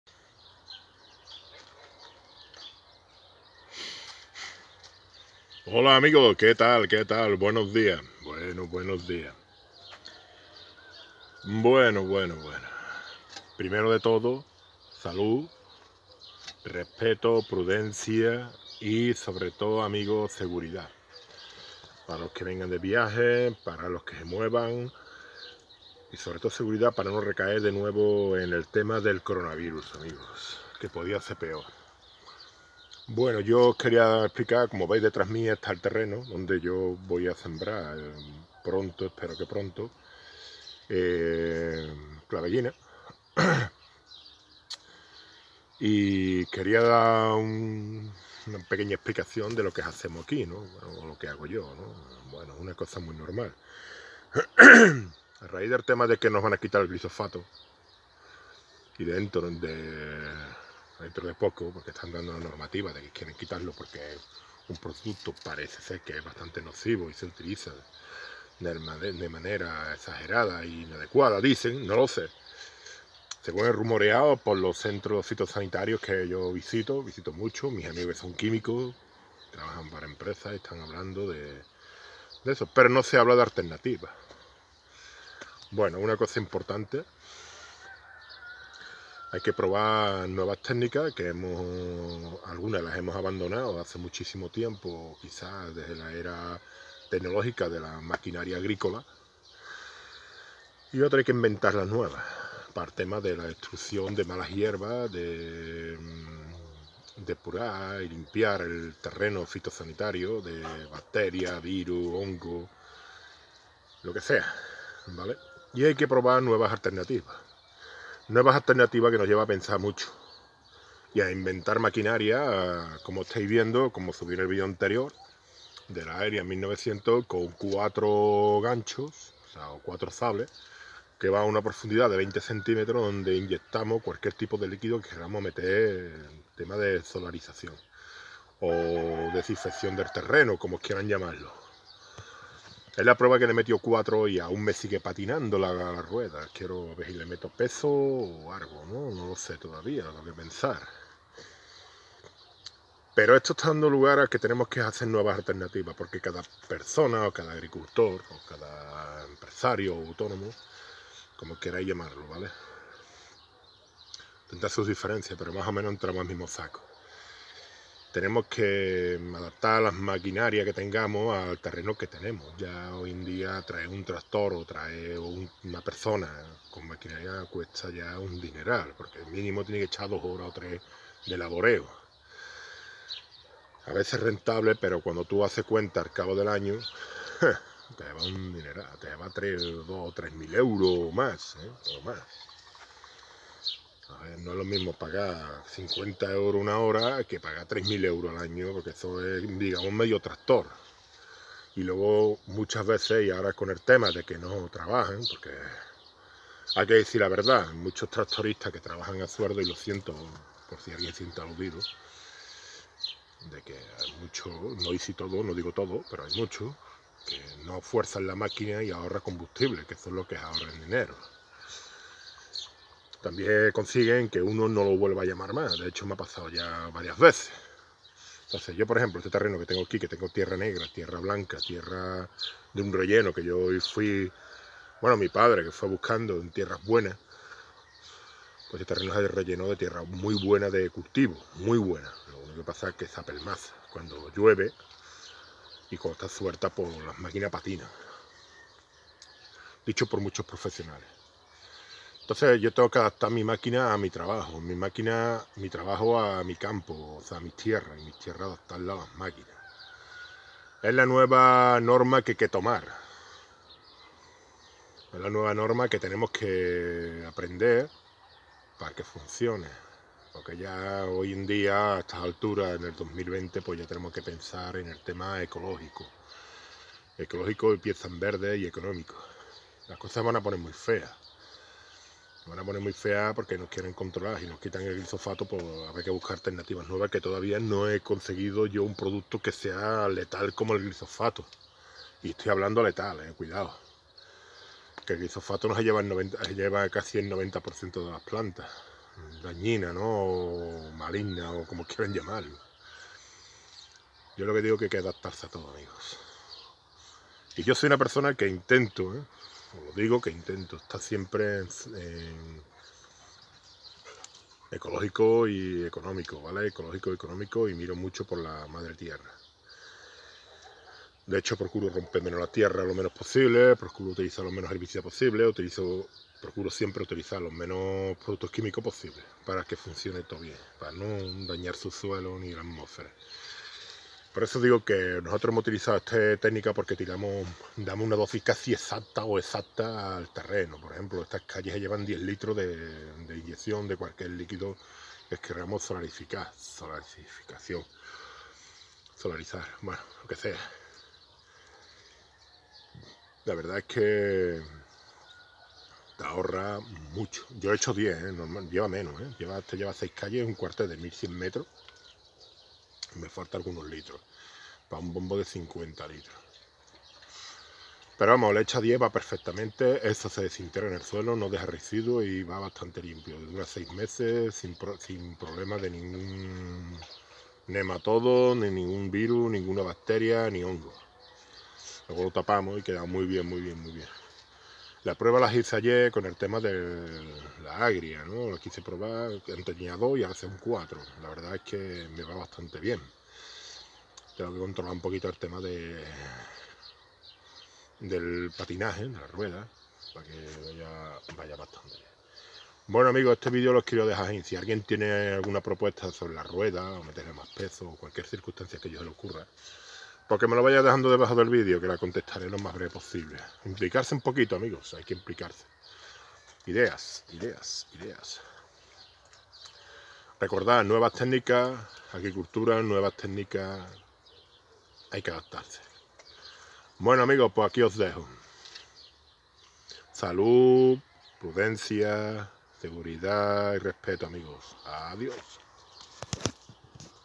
Agricultura, hay que adaptarse a las nuevas tecnicas.
Hola amigos traígo este video con una o varias razones de peso, tenemos que adaptarnos a las nuevas tecnicas de laboreo agricola, hay lo dejo, salud y seguimos.